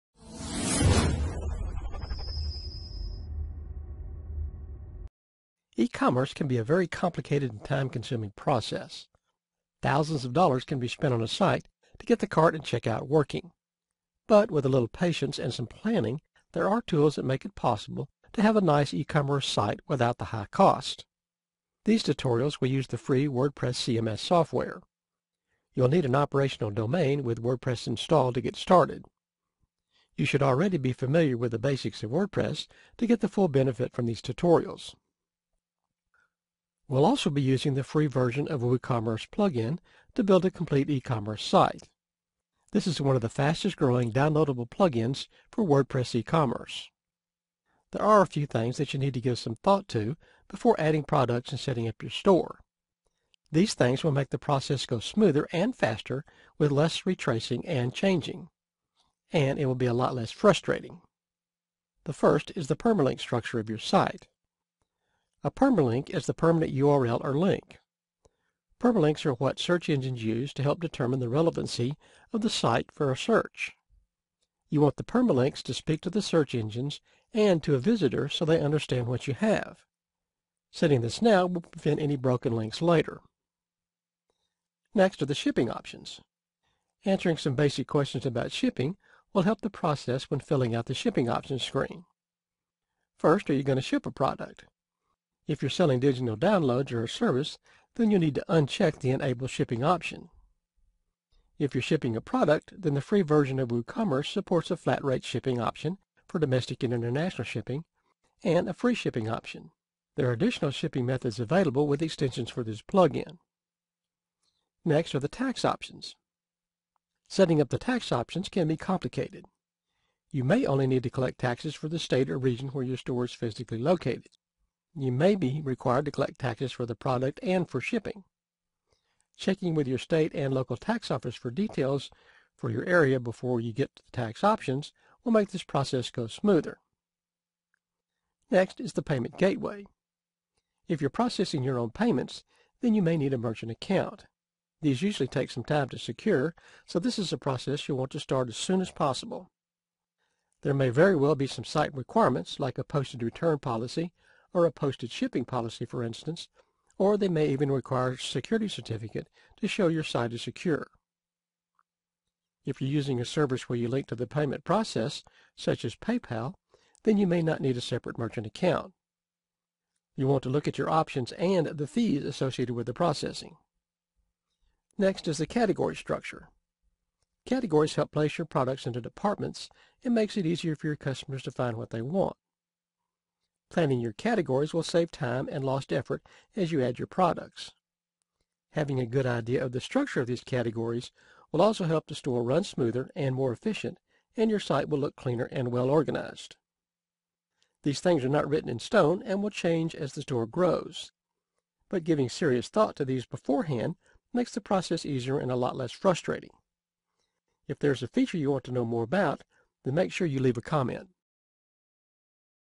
WooCommerce Plugin Tutorial - Getting Ready
WooCommerce Plugin Tutorial Series

E-Commerce can be a very complicated and time consuming process. Thousands of dollars can be spent on a site to get the cart and checkout working. But with a little patience and some planning there are tools that make it possible to have a nice e-commerce site without the high cost.

These tutorials will use the free WordPress CMS software. You will need an operational domain with WordPress installed, to get started. You should already be familiar with the basics of WordPress to get the full benefit from these tutorials.